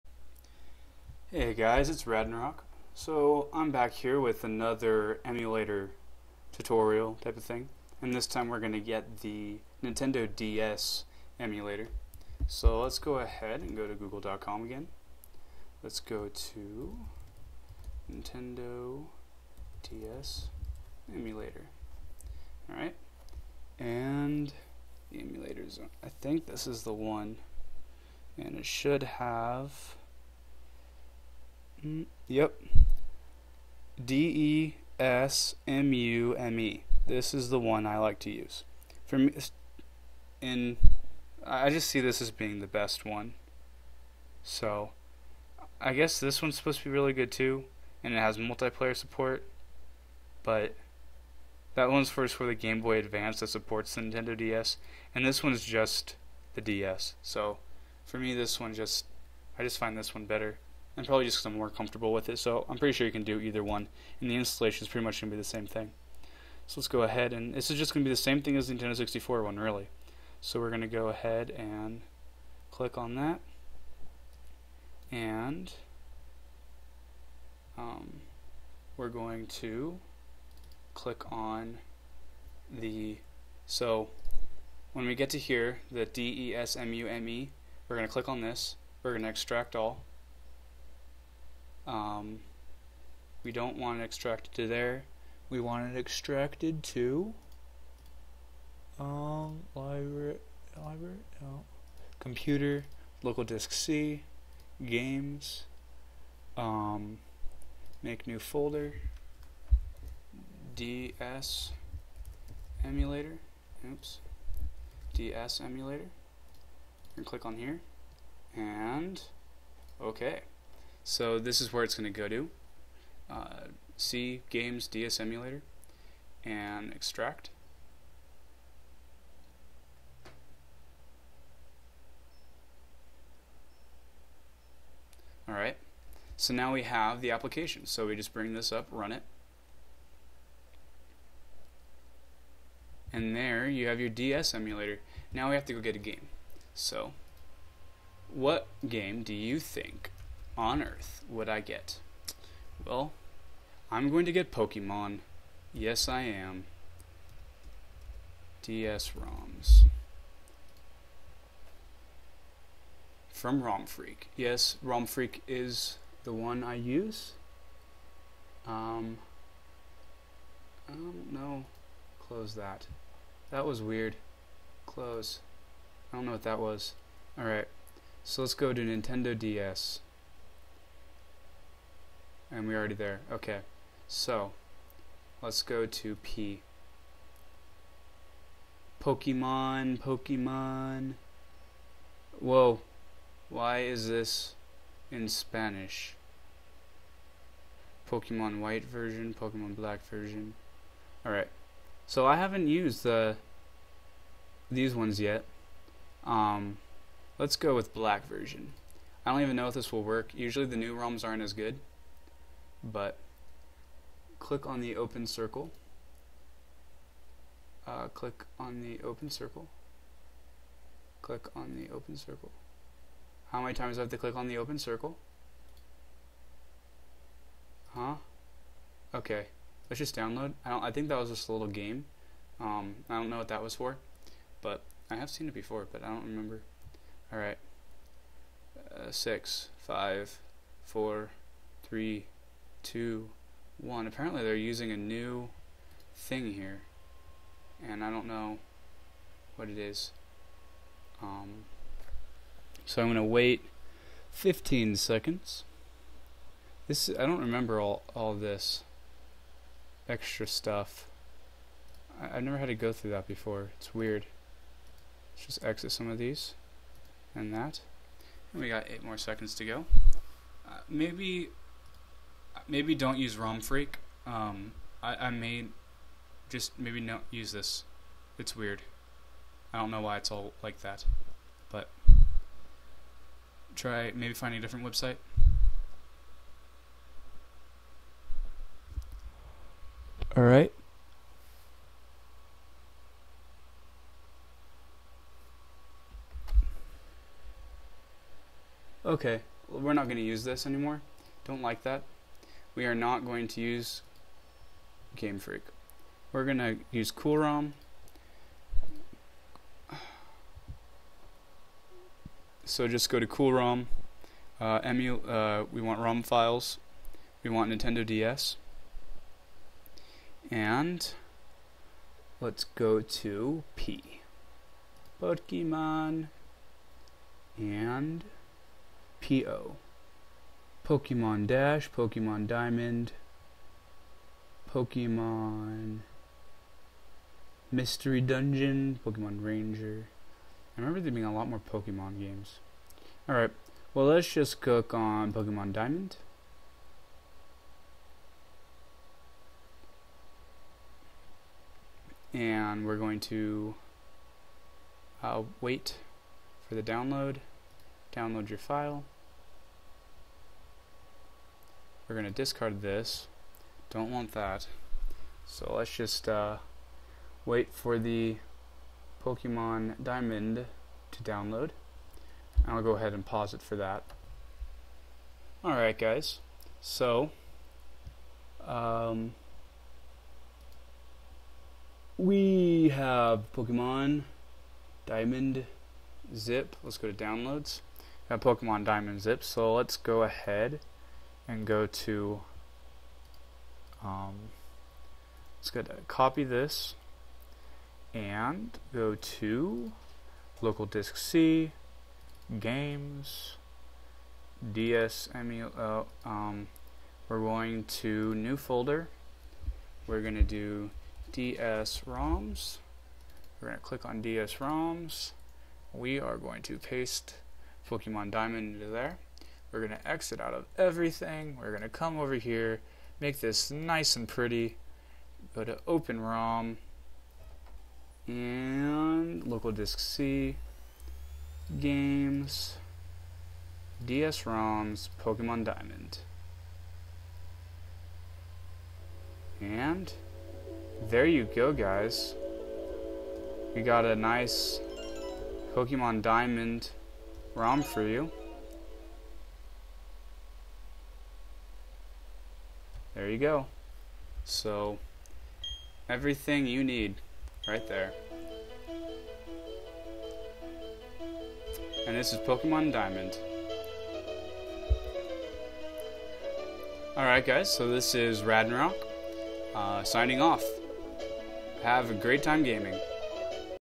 Game4orce - How to get a DS emulator and Pokemon
Going over how to download a Nintendo DS emulator and get roms. Now you can play pokemon on your PC!